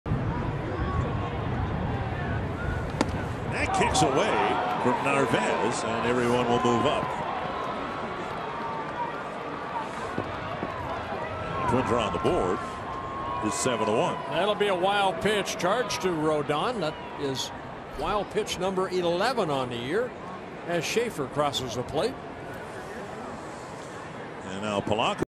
MIN@CWS: Wild pitch by Rodon plates Schafer
Logan Schafer comes in to score on the wild pitch by Carlos Rodon in the top of the 6th inning